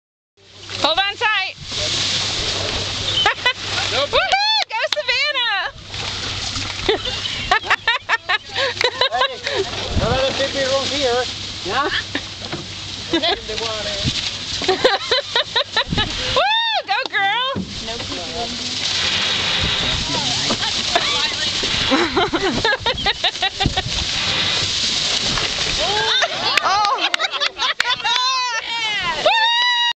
Savannah on the Spinnaker!
My friend, Savannah, takes a ride on the Spinnaker in Cancun.